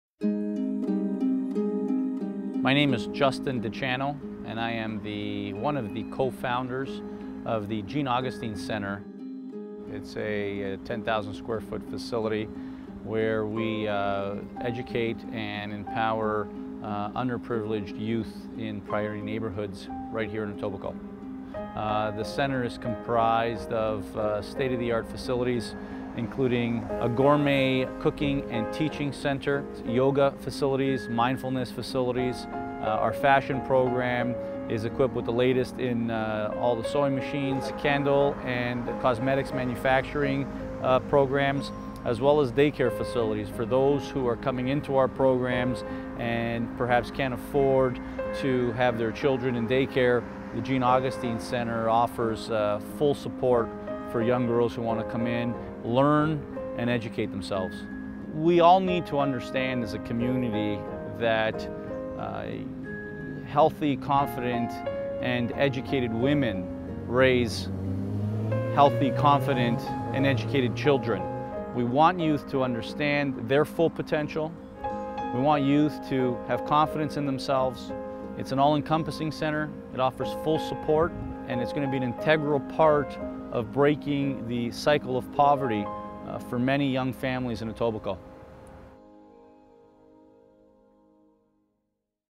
Making Idea's Happen - Justin Di Ciano talks about the Jean Augustine Center
The Jean Augustine Centre has been established to ensure young women get the support and guidance they need to make the right decisions. The Centres programs focus on education, nutrition, mindfulness and real-world job training.